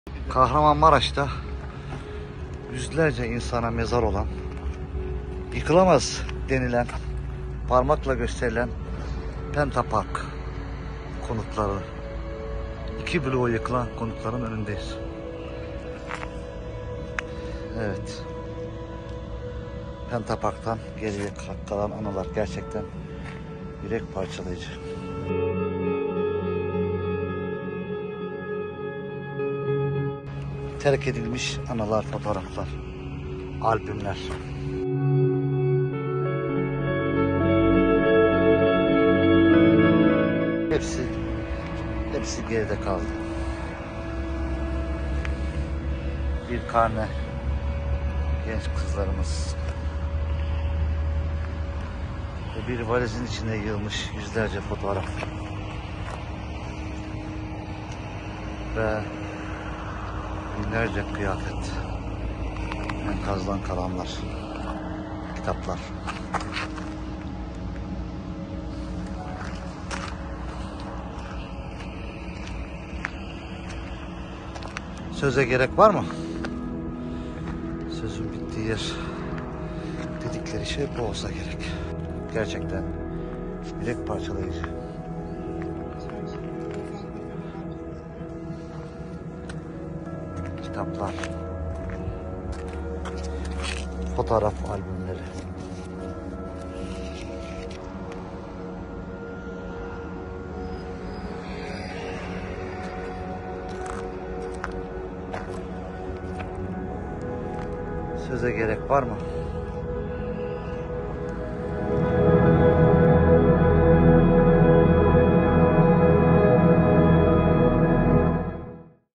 SÖZÜN BİTTİĞİ YER! YIKILMAZ DENİLEN SİTENİN ENKAZINDAN ÇIKANLAR DUYGULANDIRDI!
Kahramanmaraşta şehrin en lüks siteleri arasında gösterilen Pentapark isimli konut, deprem sonrası tamamen tuzla buz oldu. O lüks siteden geriye sadece anılar, fotoğraflar ve eşyalar kaldı. İşte yıkılmaz denilen Pentapark’tan geriye kalanlar…

▶️ Dosya-Haber: shorturl.at/impz9

shorturl.at/GHORU